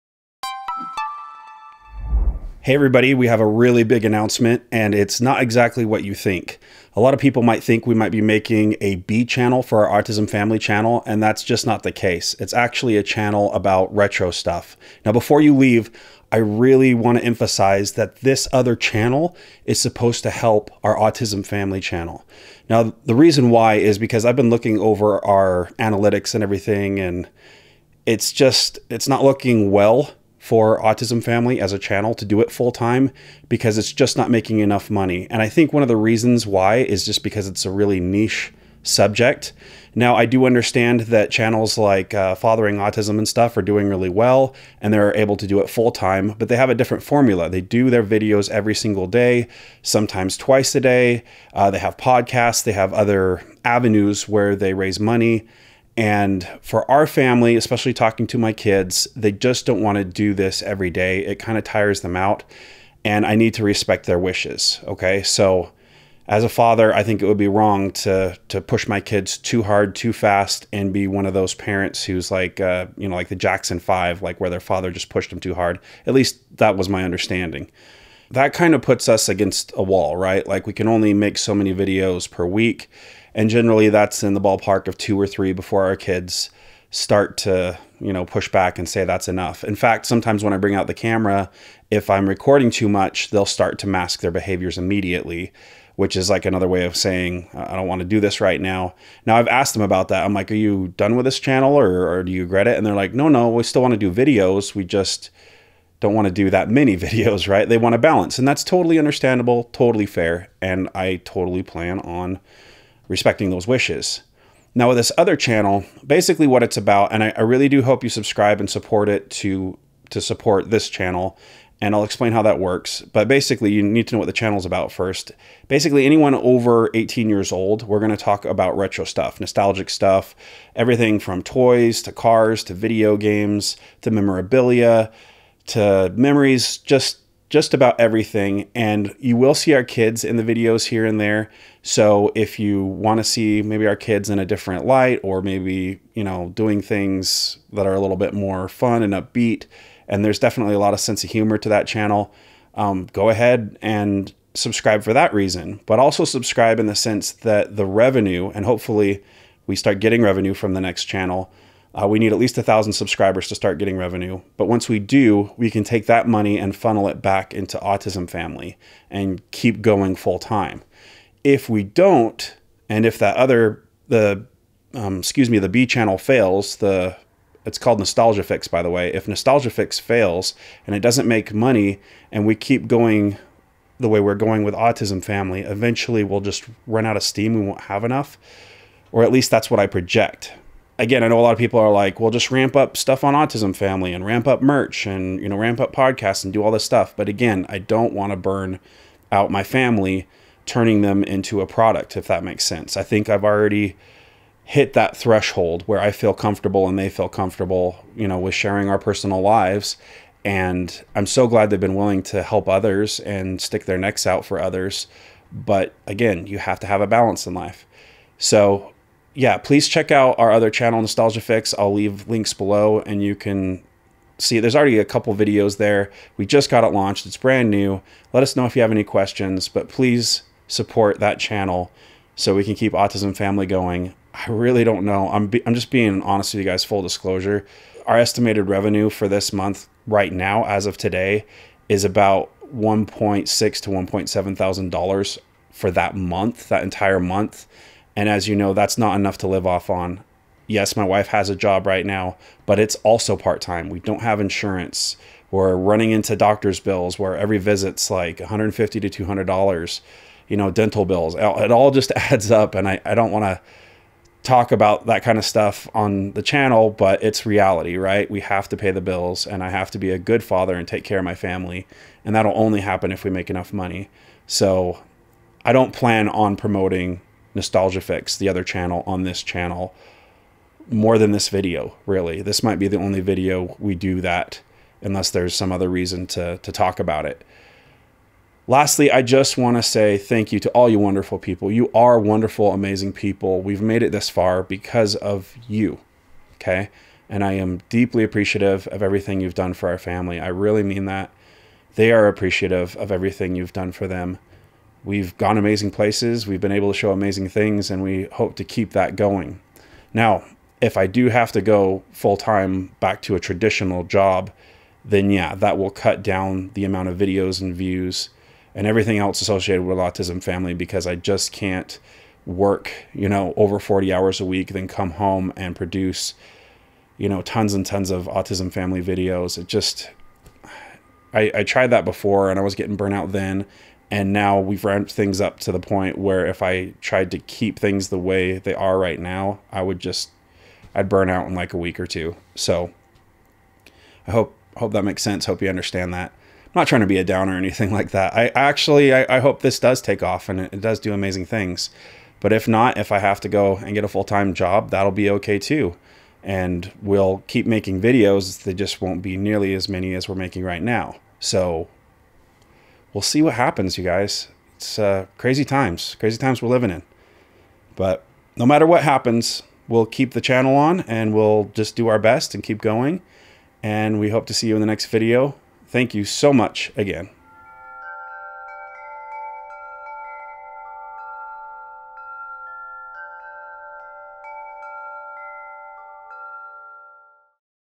We Started a New YouTube Channel
Here's the link to the new channel:
This is a huge deal for us and I've been working on this channel in the background getting it ready for launch. But there's more to it than that. This other channel could potentially help keep our Autism Family channel going full time. It's really hard to predict the future, but we hope you'll subscribe to our new channel and support our endeavors to keep doing YouTube full time so Autism Family can succeed.